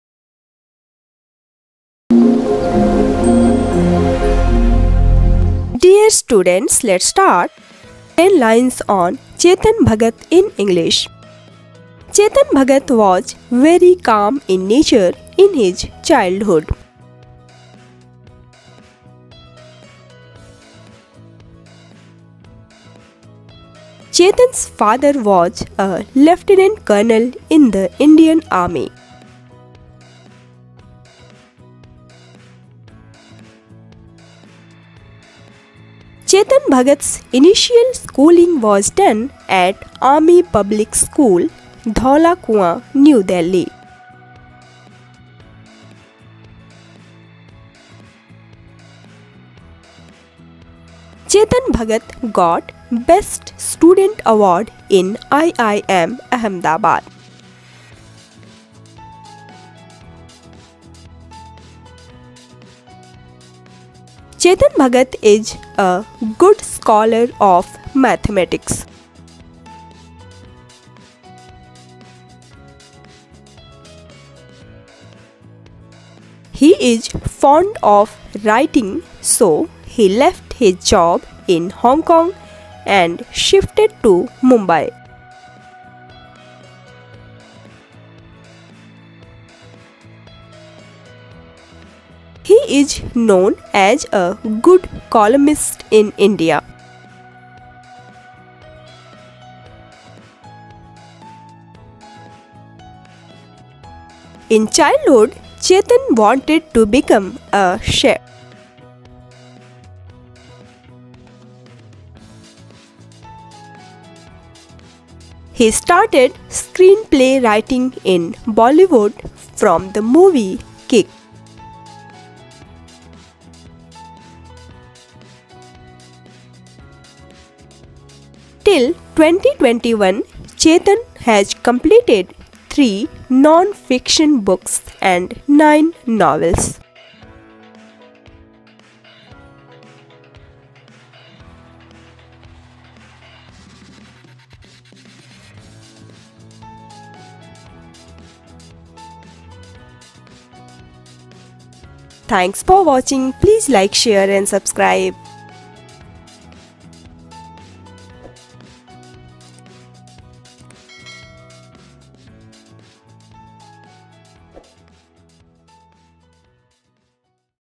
10 Lines on Chetan Bhagat in English || Teaching Banyan
10 Lines on Chetan Bhagat in English. Find here ten points on Chetan Bhagat for School and College Students. These lines on Chetan Bhagat are very easy and written using simple words in English.